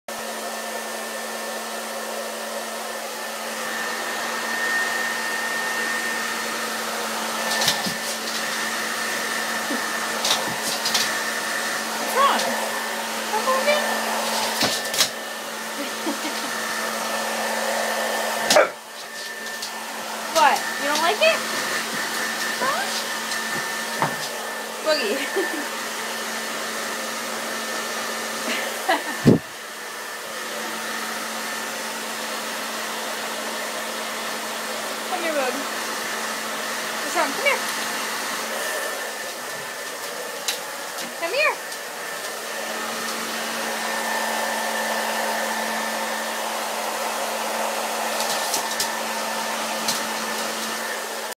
Pitbull and Hair Dryer
Boogie hates hair dryers... I think.